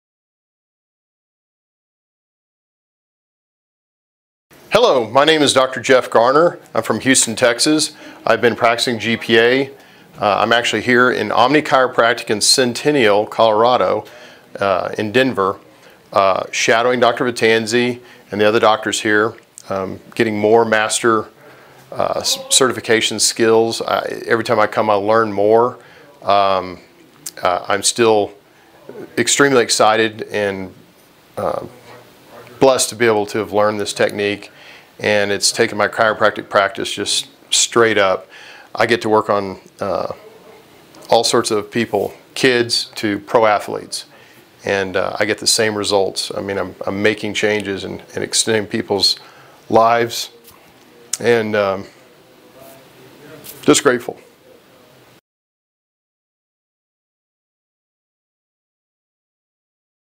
Dr. Jeffrey Garner talks about Gravitational Pattern Alignment
Dr Jeffrey Garner, talks about Gravitational Pattern Alignment training with Dr. Vetanze.